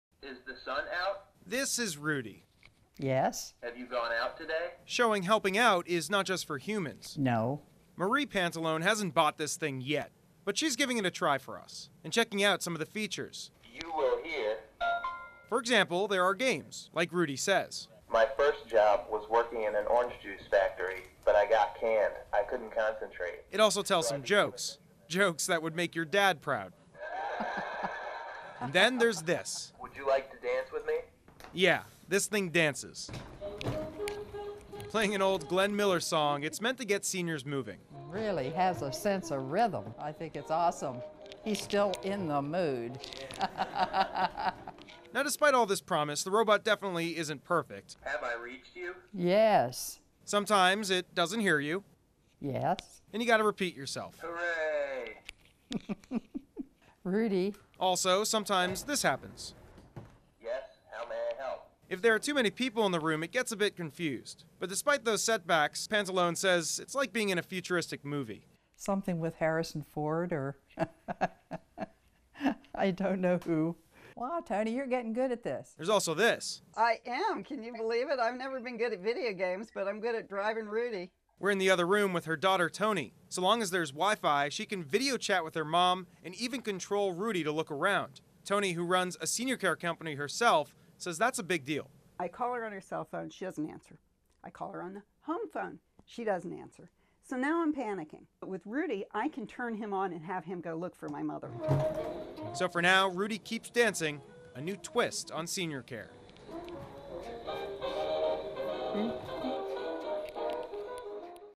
'RUDY' Robot acts as virtual caregiver
A brand new robot brings out the gentle side of R2D2. The robot, being called "RUDY" acts as a care giver for the elderly. WUSA9 stopped by the Fairfax home of Marie Pantalone, as she gave the robot a try.